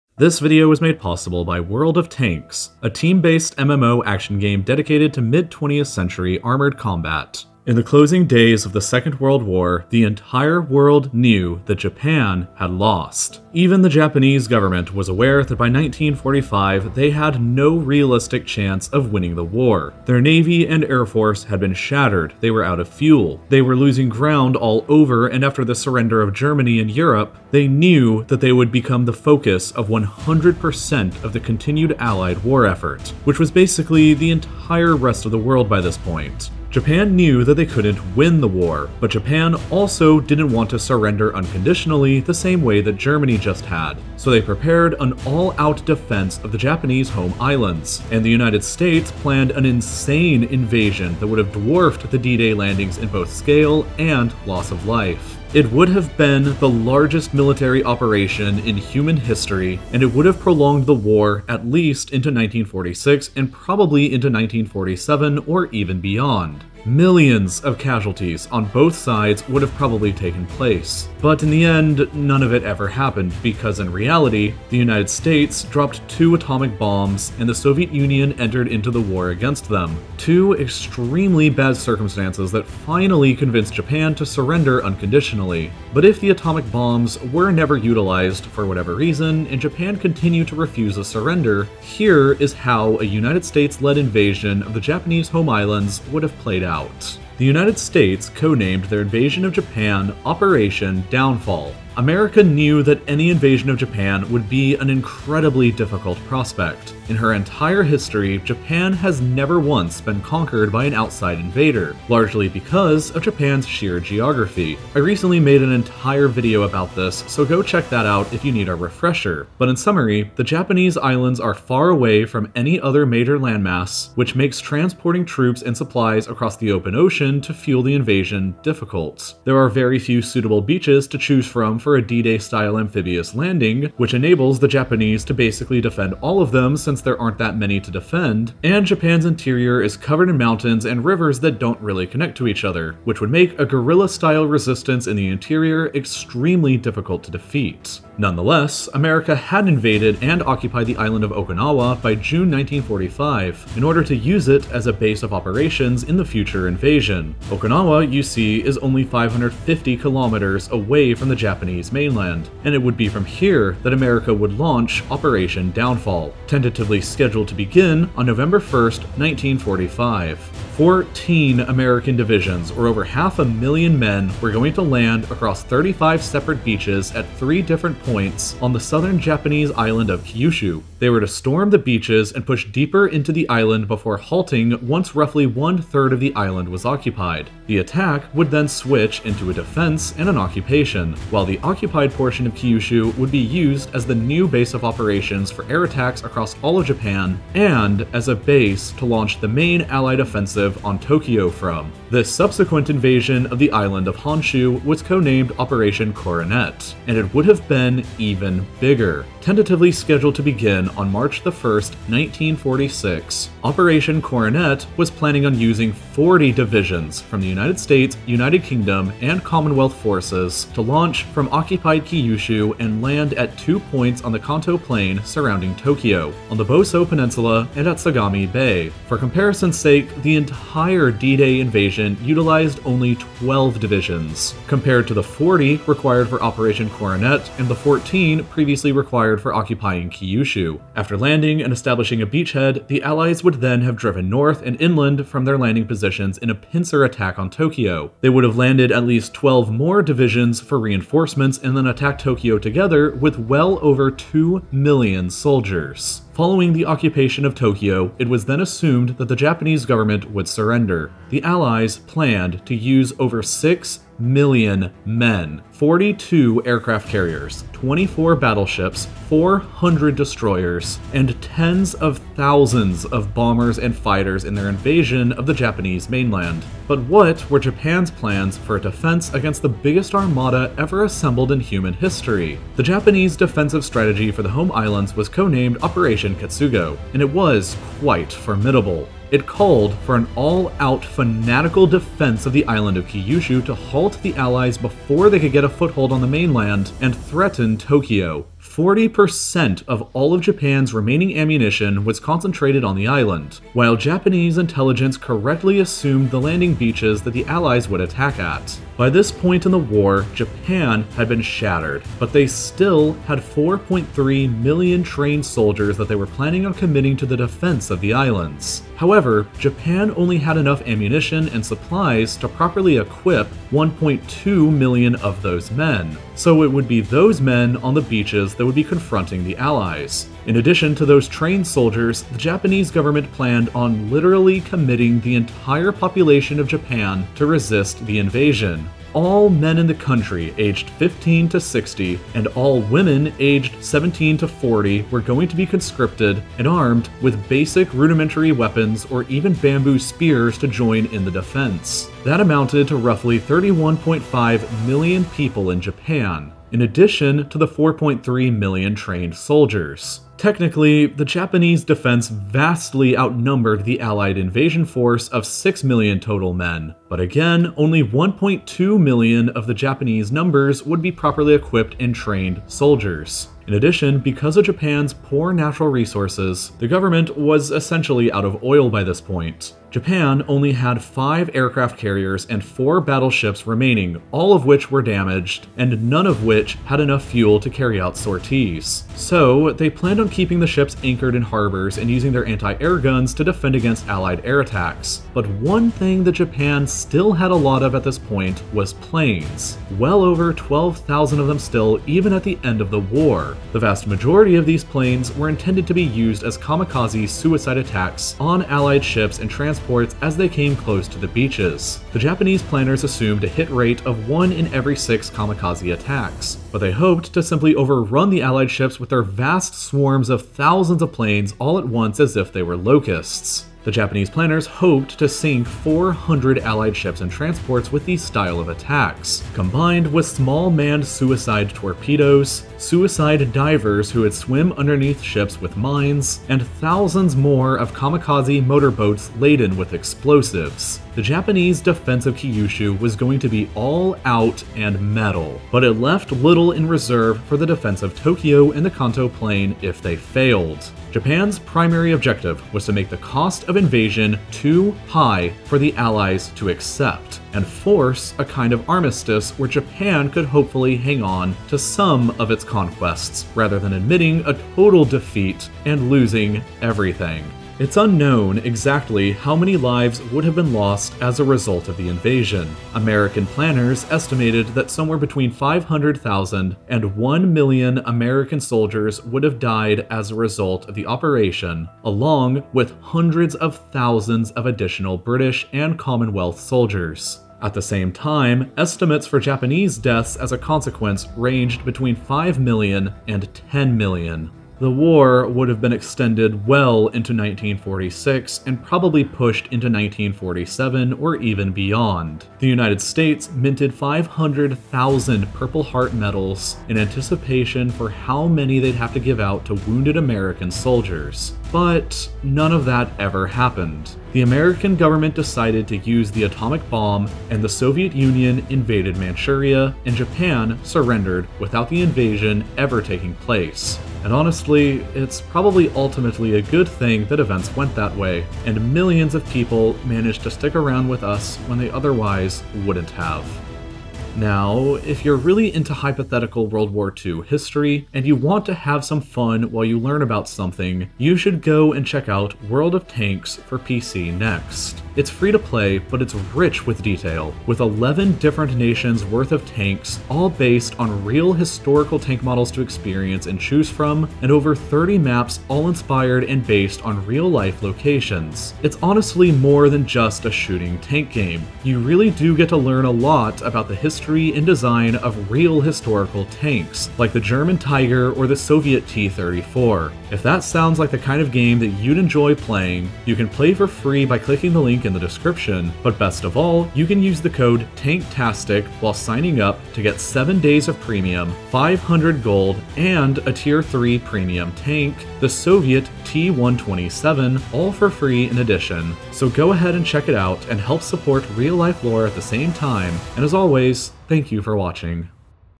The Insane American Plan to Invade Japan in 1945
Play World of Tanks for free and us code TANKTASTIC to get 7 days premium, 500 gold and a premium tank also free

The American plan to invade Japan in 1945 was known as Operation Downfall. It included 2 separate operations inside of it. Operation Olympic was the planned invasion of the island of Kyushu, while Operation Coronet was the planned invasion of the Kanto Plain. This video is a brief overview of the entire operation.